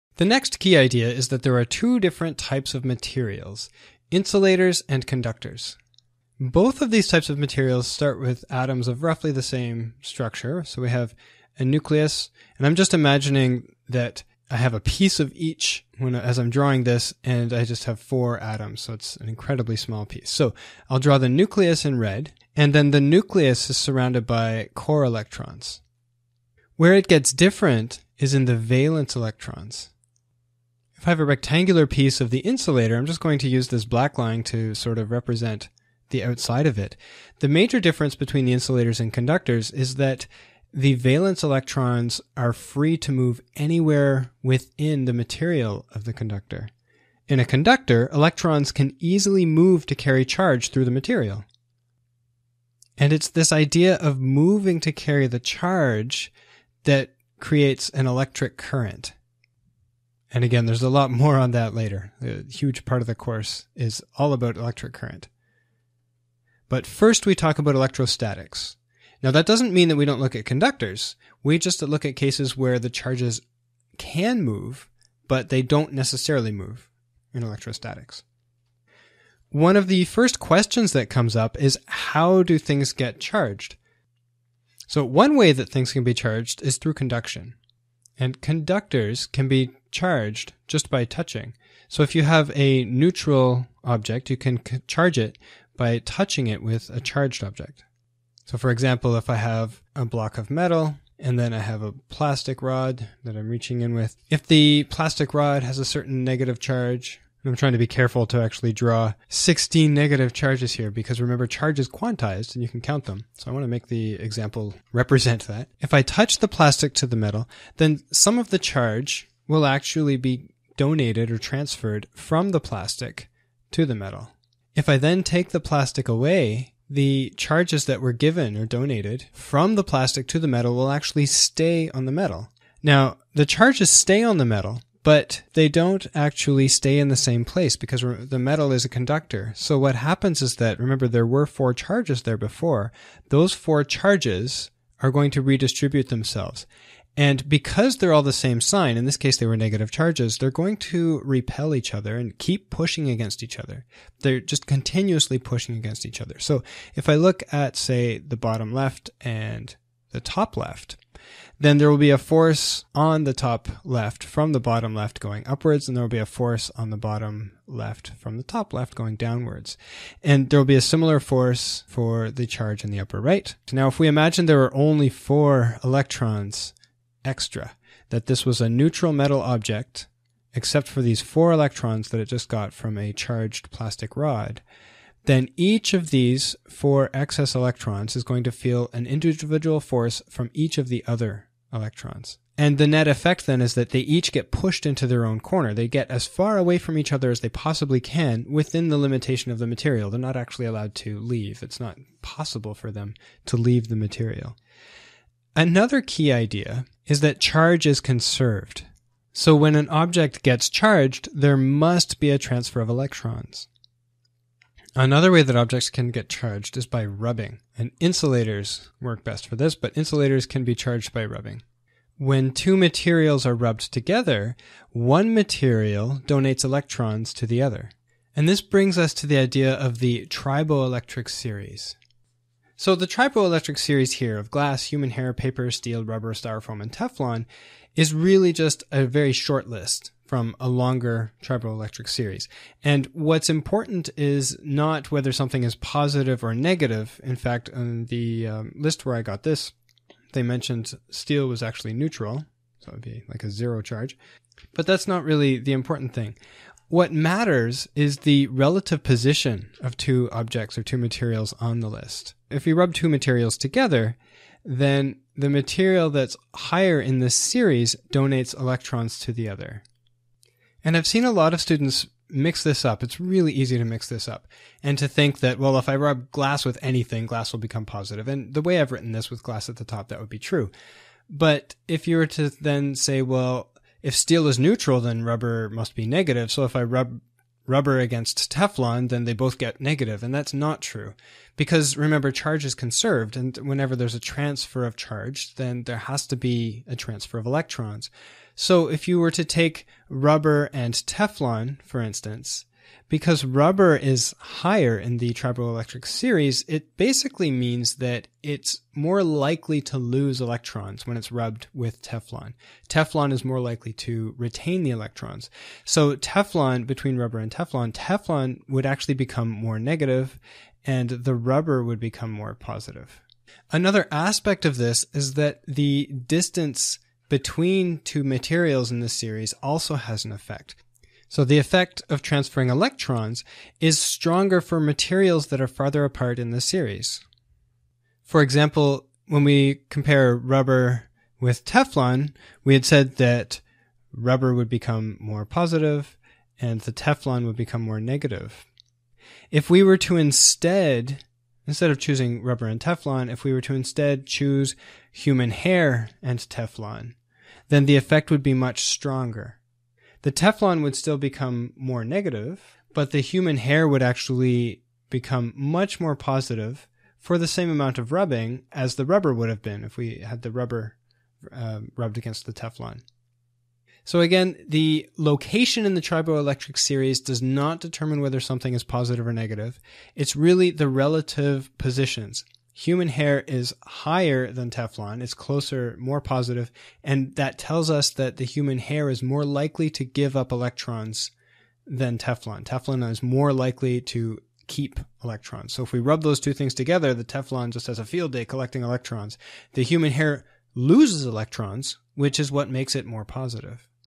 E&M Course 1-2: Insulators, Conductors, and How Objects Become Charged
Conductors are quite different from insulators, and this lecture quickly shows you how.  I also show you how objects can become charged, including a description of how to use the triboelectric series to determine which object will become positive (or negative) when two materials are rubbed together.